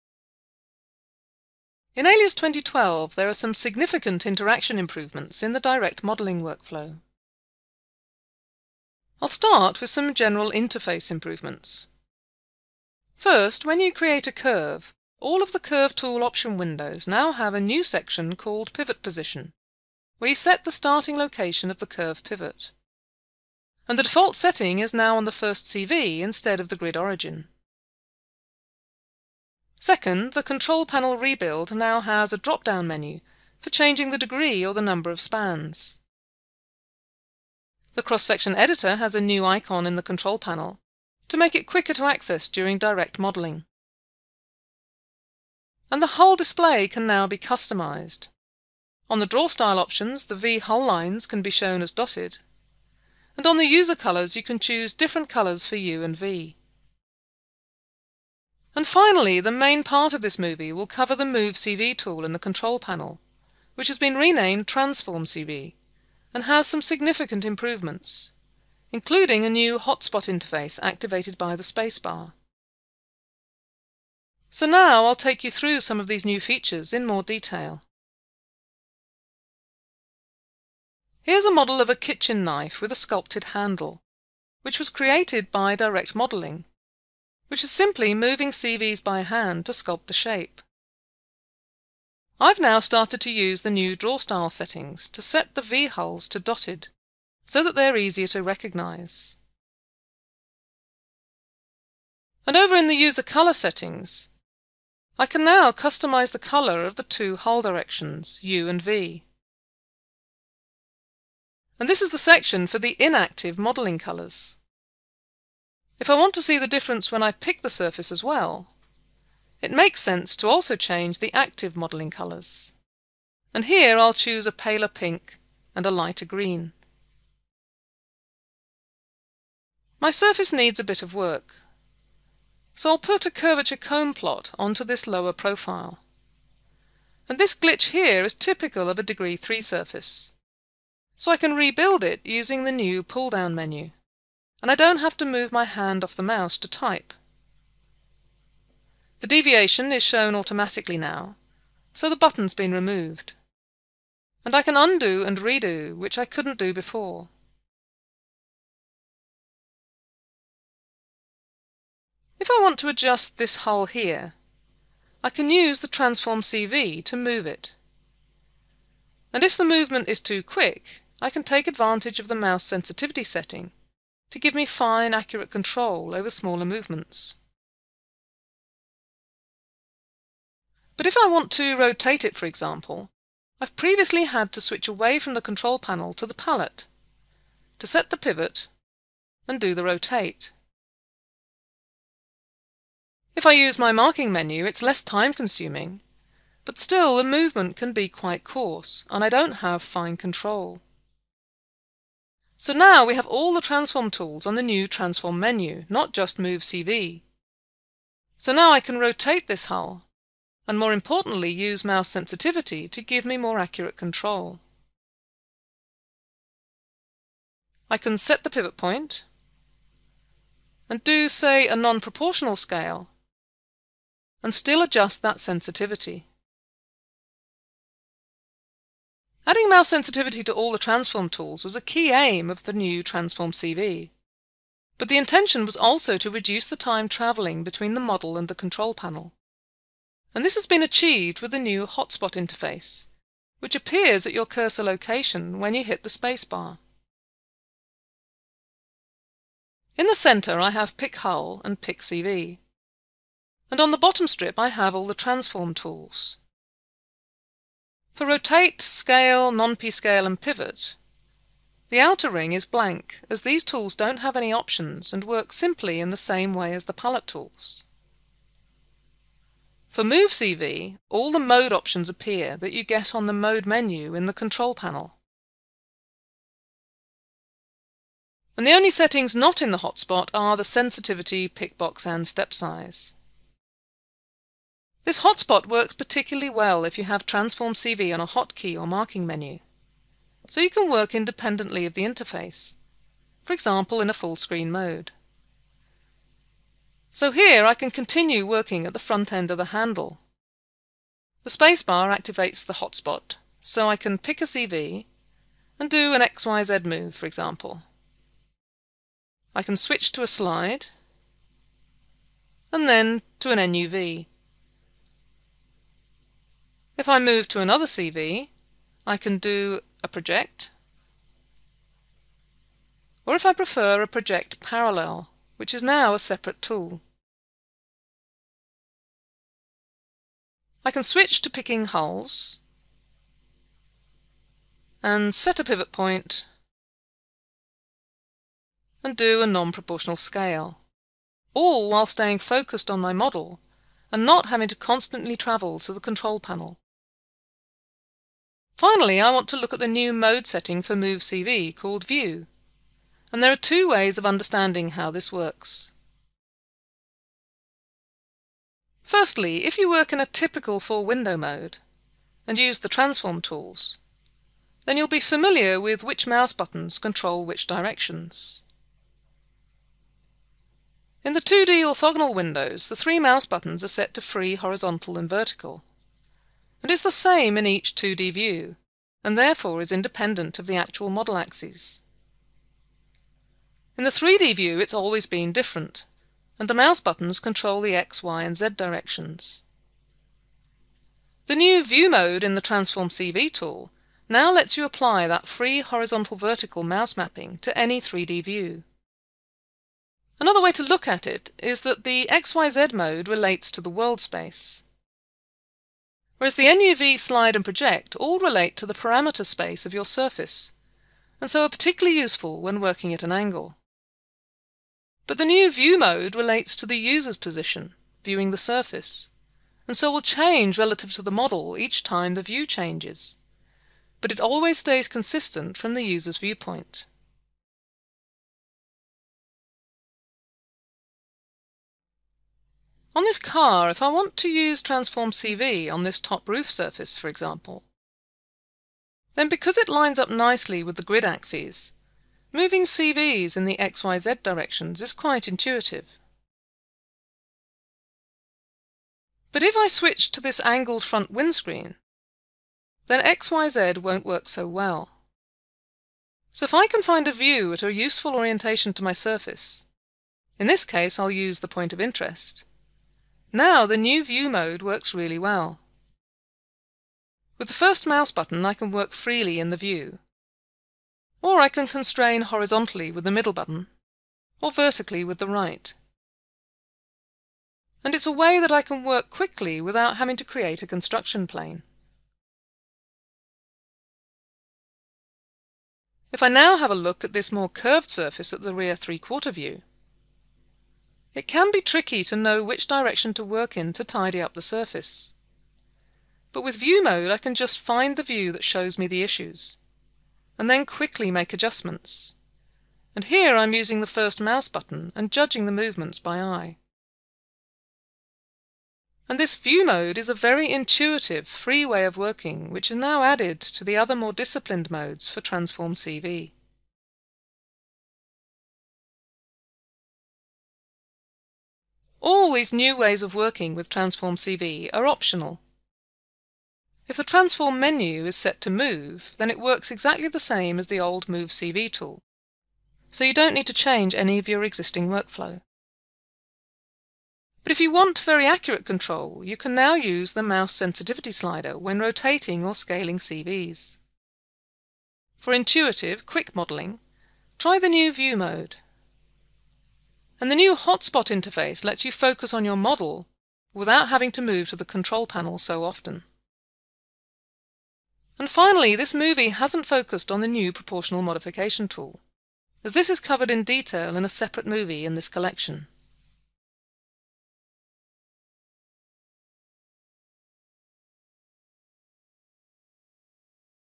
Autodesk Alias 2012 Transform Control Vertices
Indepth Overview of the updated Transform Control Vertices tool inside Autodesk Alias 2012